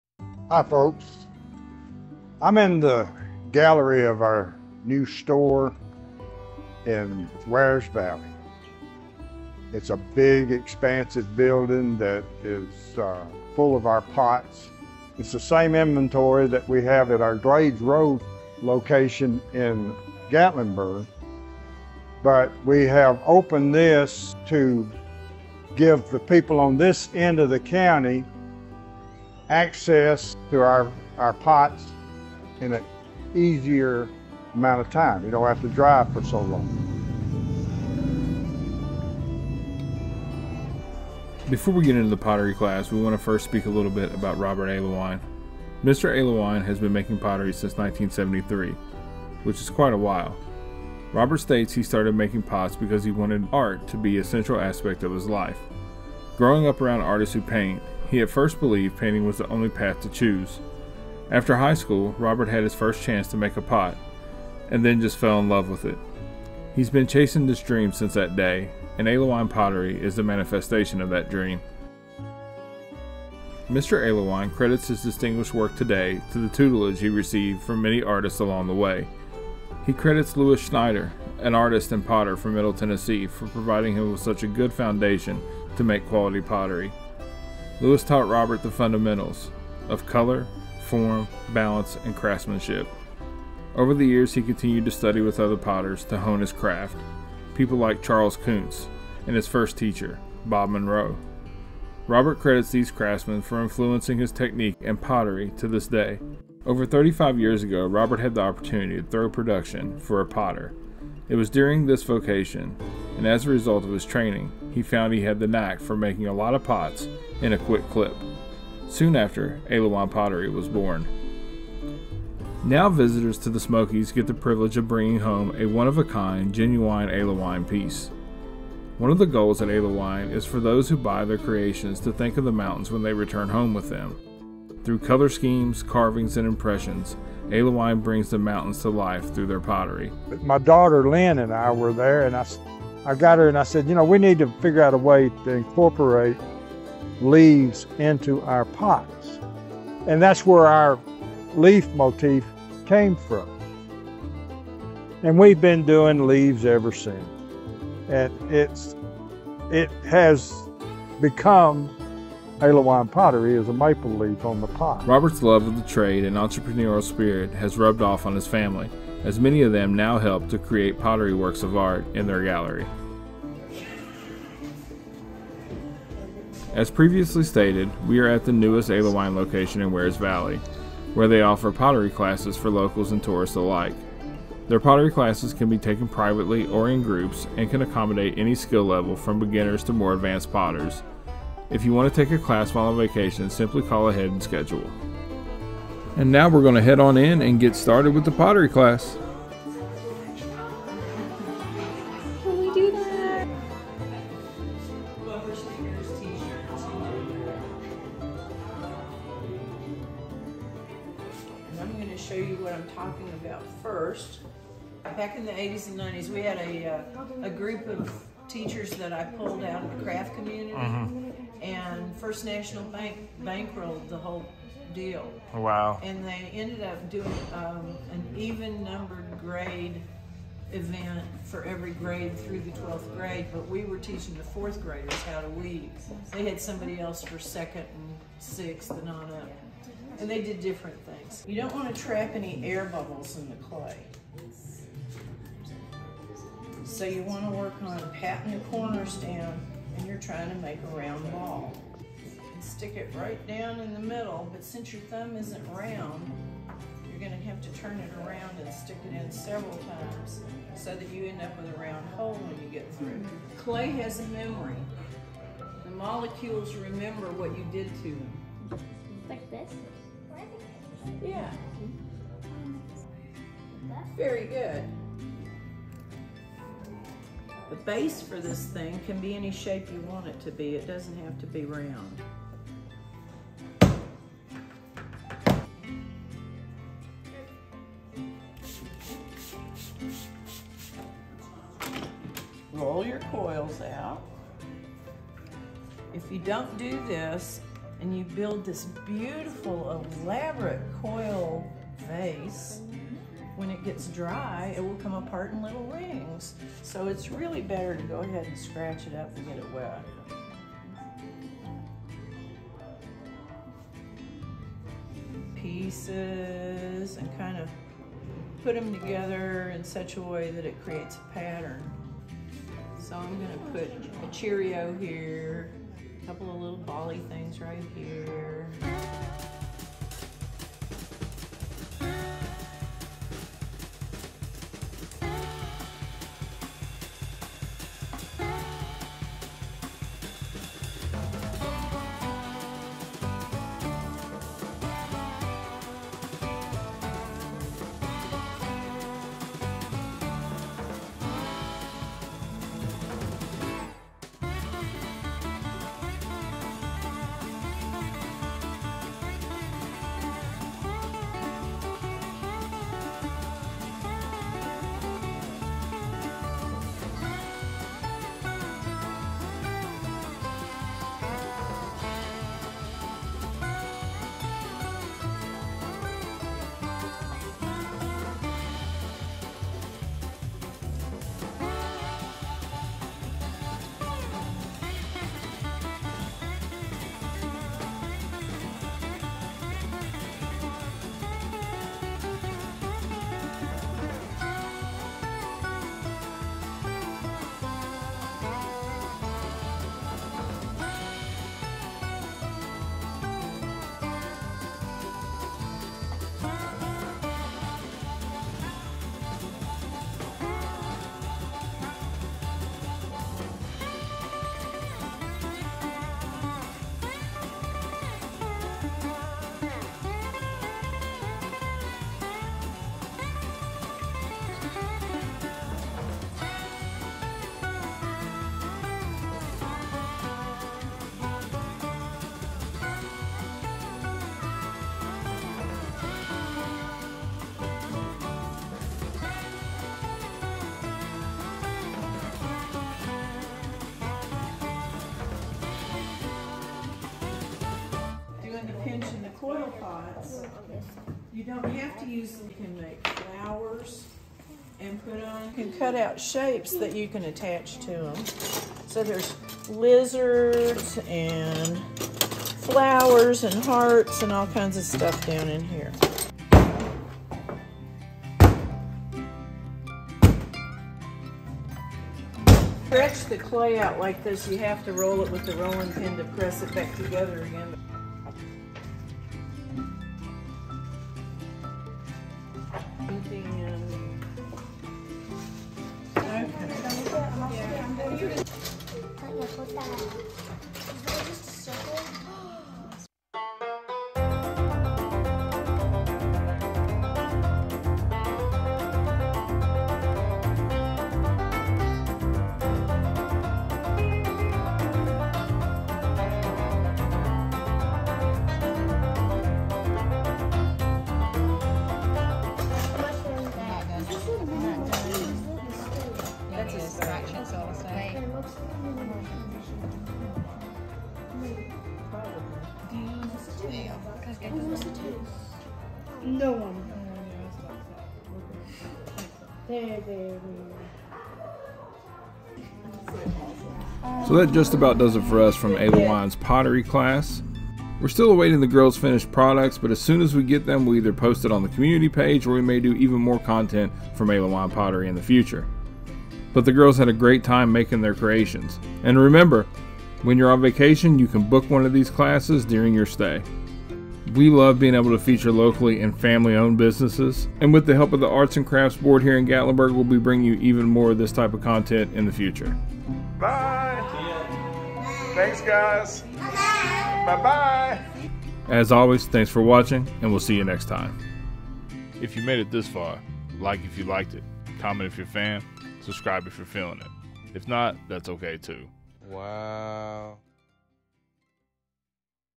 THE ICONIC ALEWINE POTTERY |We Take Their New Pottery Classes| + A Brief History of Alewine
The girls are taking pottery classes at Alewine Pottery. This content also features a brief history of Alewine and Master Potter Robert Alwewine.

Gatlinburg Tennessee 37738